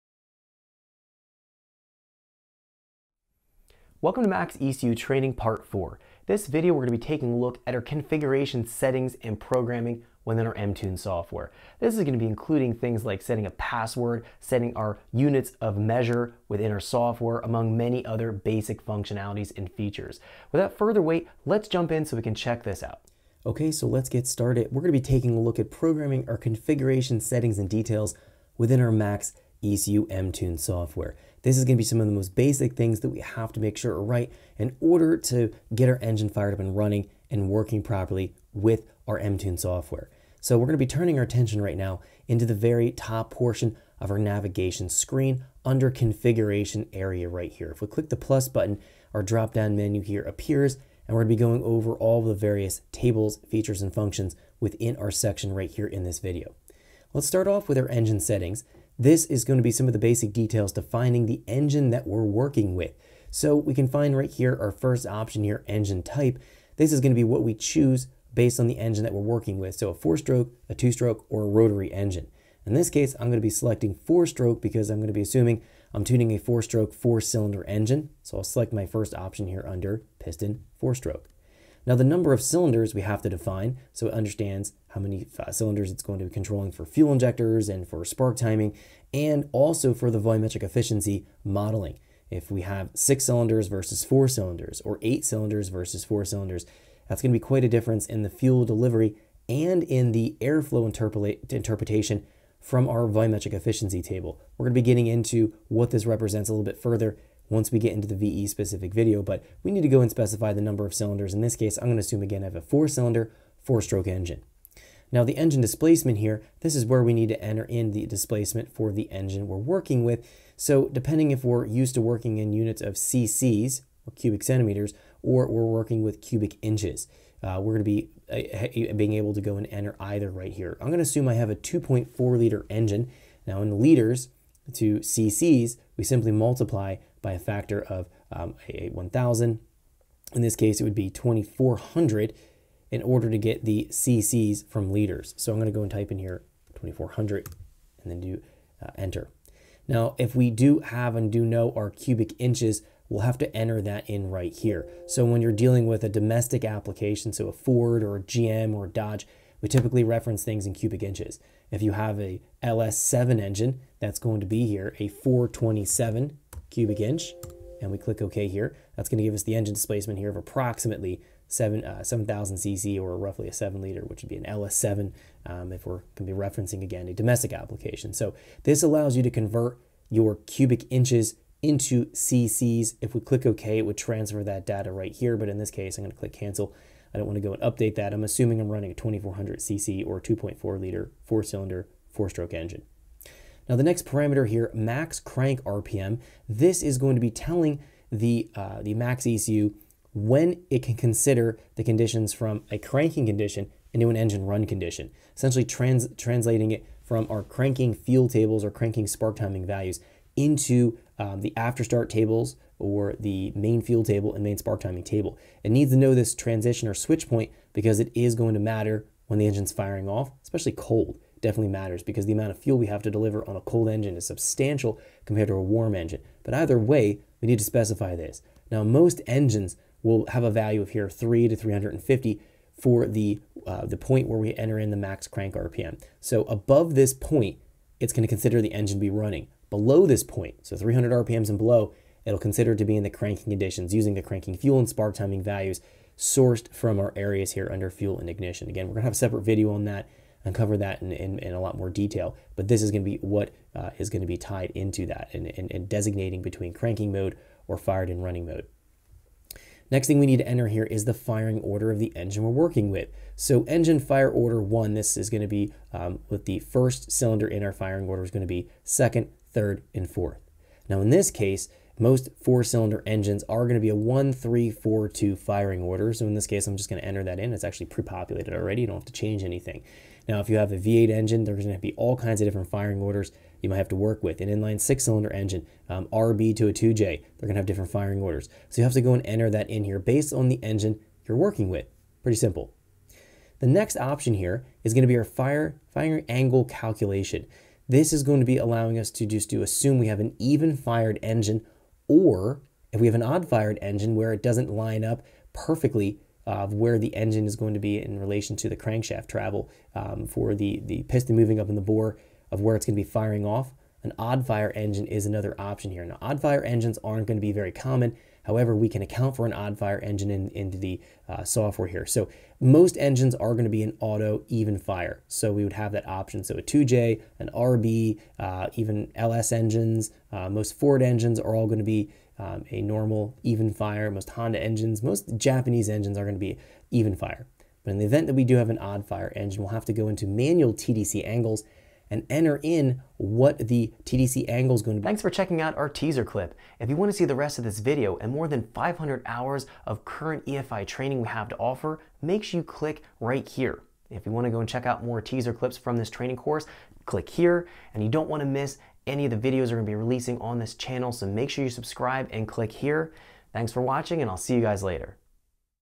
MaxxEcu Training Part 4: Configuration Settings | Evans Performance Academy
MaxxEcu Training Part 4: Configuration Settings walks you through basic programming details using the MTune software such as engine configuration, password protection, firmware updating, and more.